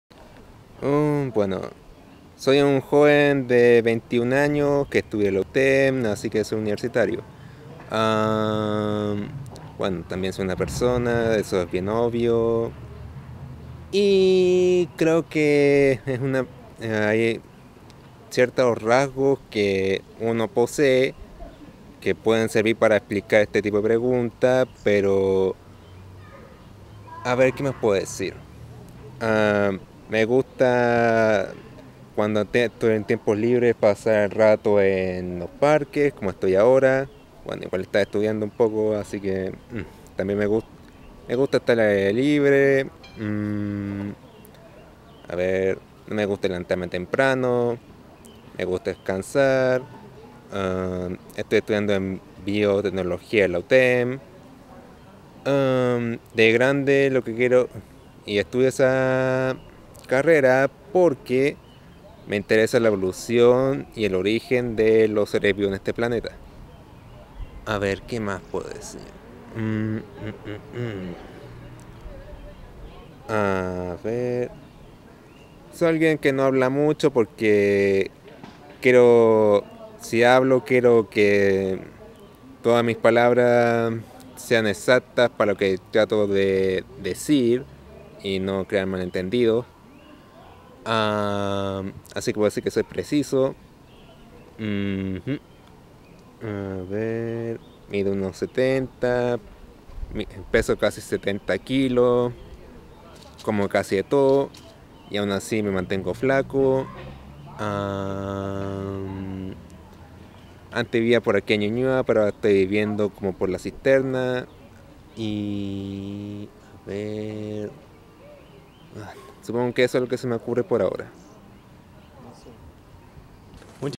Me interesa la evolución y el origen de los seres vivos en este planeta...
¿Quiénes somos? es una serie de micro relatos generados de manera espontánea, con la honestidad de quien es sorprendido en la calle ante una pregunta: ¿Quién eres?

Un experimento audiovisual que, gracias a la apertura de los entrevistados, hoy se convierte en un espacio de búsqueda, reflexión y debate acerca de los elementos que nos definen como personas.